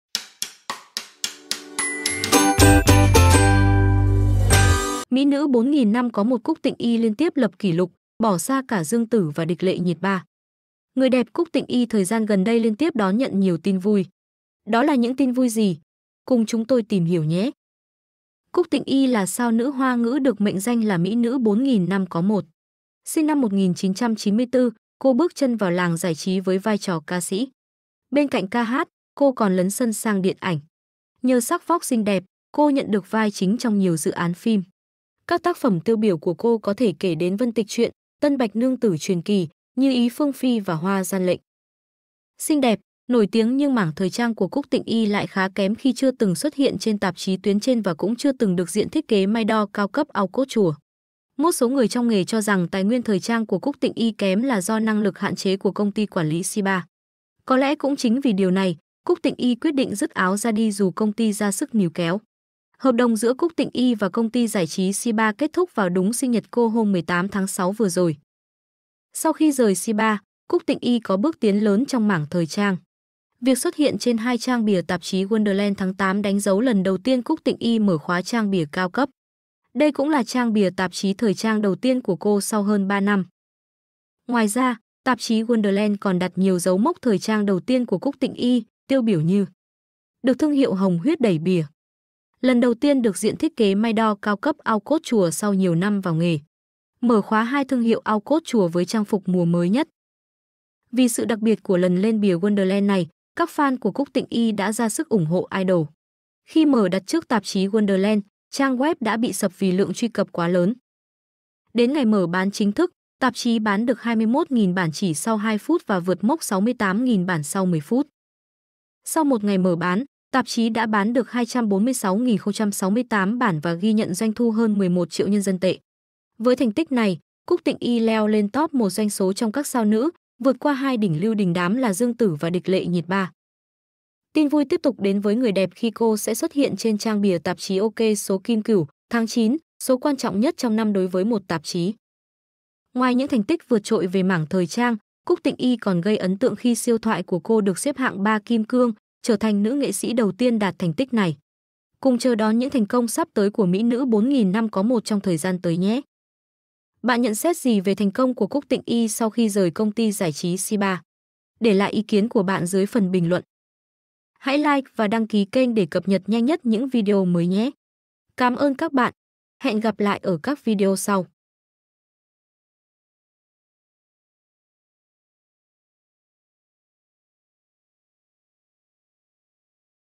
'Mỹ nữ 4000 năm có một' Cúc Tịnh Y liên tiếp lập kỷ lục, bỏ xa cả Dương Tử và Địch Lệ Nhiệt Ba
'Mỹ nữ 4000 năm có một' Cúc Tịnh Y liên tiếp lập kỷ lục đáng ngưỡng mộ, thậm chí bỏ xa cả Dương Tử và Địch Lệ Nhiệt Ba, nữ ca sĩ Lưu Vũ Hân...

Người đẹp Cúc Tịnh Y thời gian gần đây liên tiếp đón nhận nhiều tin vui. Đó là những tin vui gì. Cùng chúng tôi tìm hiểu nhé.